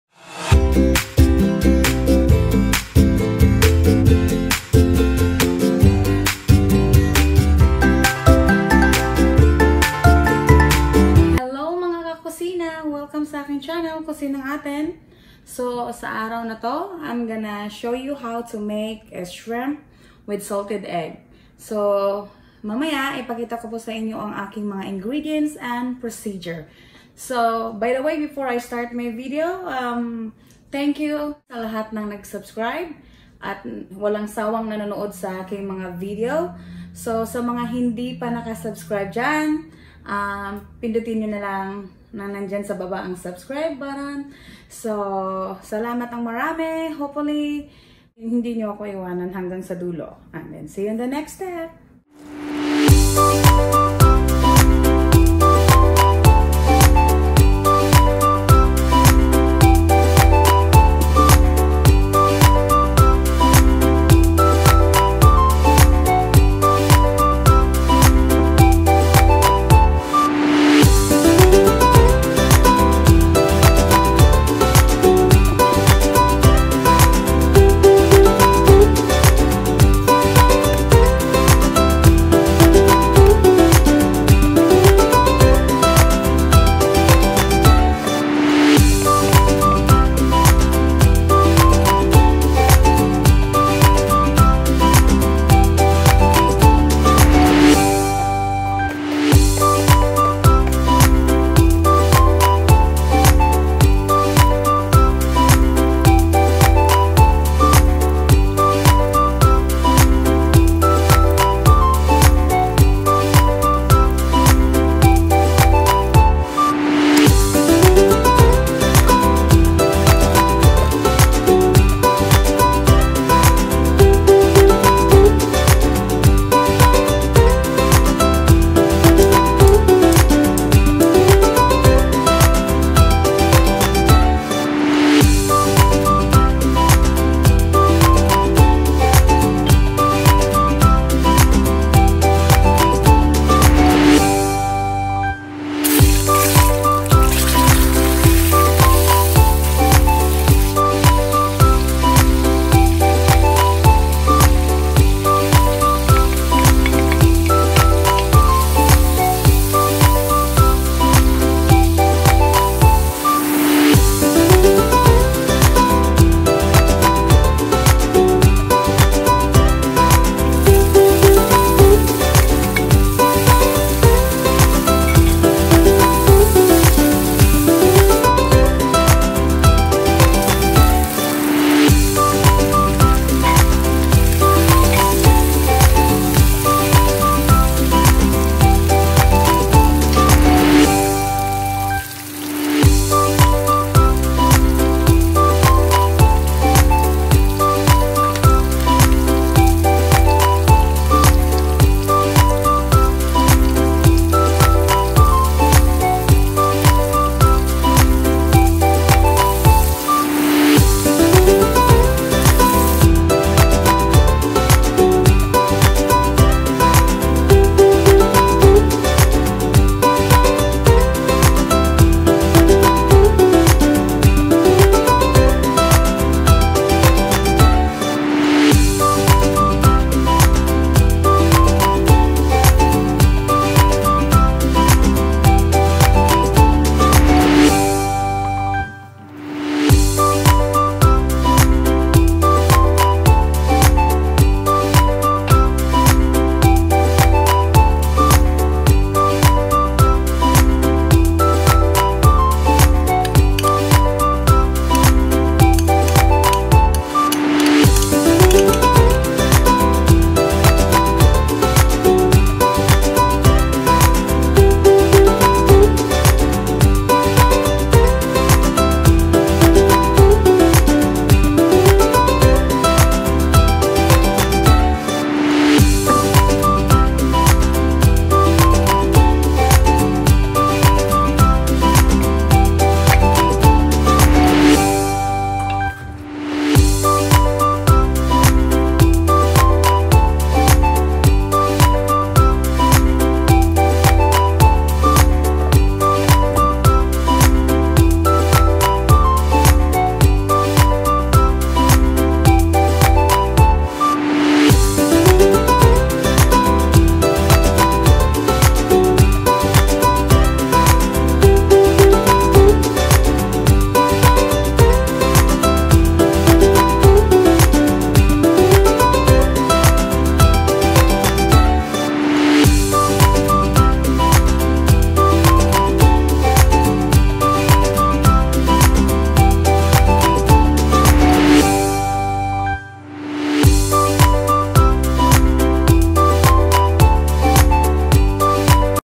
How to cook Shrimp with Salted Egg | Kusinang Atin
Here are the list of Ingredients:
- 2lbs Shrimp
- 1 whole Garlic
- Onion
- Salt & Pepper
- Butter
- 1 can heavy cream
- Cooking Oil
- Water

Thank you guys for Watching and Supporting my channel.